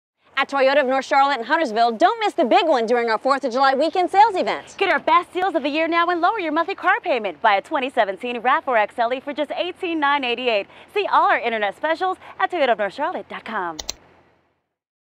You’ll love our HUGE used car deals at Toyota of N Charlotte
A big sale calls for big deals – get our biggest yet on used Toyota today during the Big One!